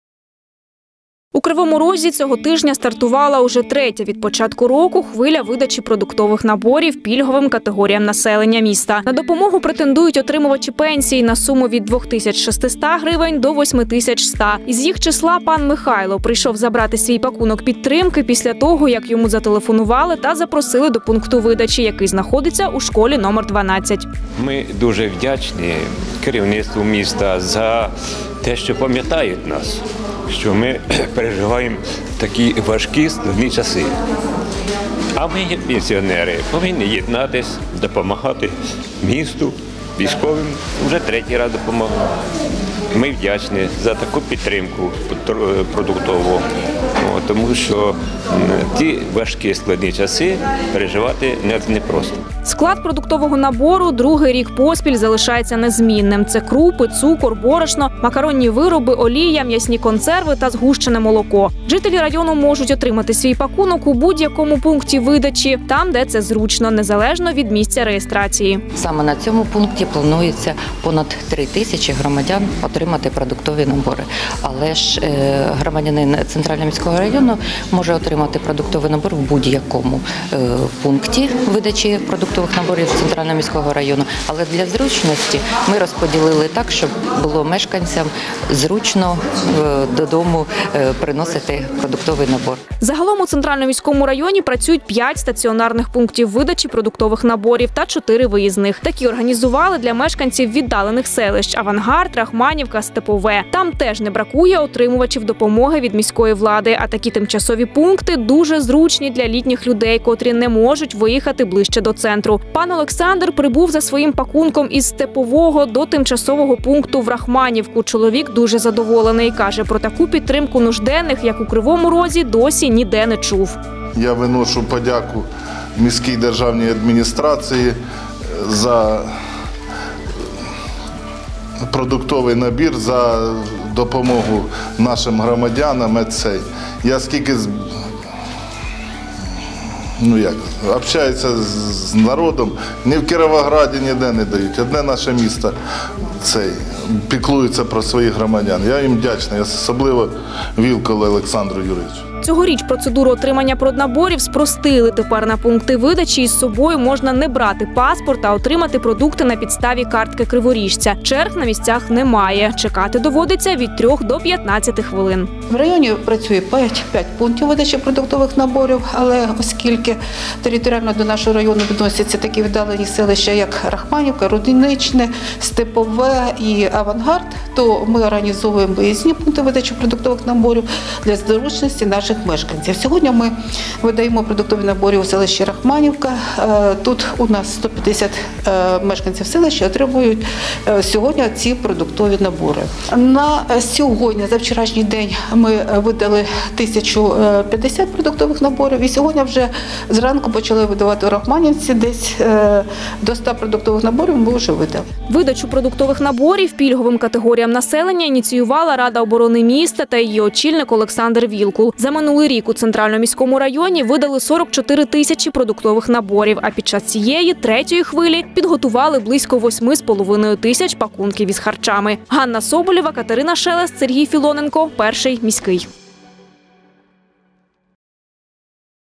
Понад 8,5 тисяч мешканців Центрально-Міського району отримають допомогу від Ради оборони
Більше 8,5 тисяч отримувачів. Саме стільки продуктових наборів у Центрально-Міському районі планують видати під час третьої цього року хвилі, ініційованої Радою оборони міста. Вміст пакунків – традиційний – продукти тривалого зберігання. Для зручності людей організували як стаціонарні так і виїзні пункти видачі – для мешканців віддалених селищ. Як проходить процес отримання допомоги у Центрально-Міському районі та що говорять про такі ініціативу міської влади люди, знають наші кореспонденти.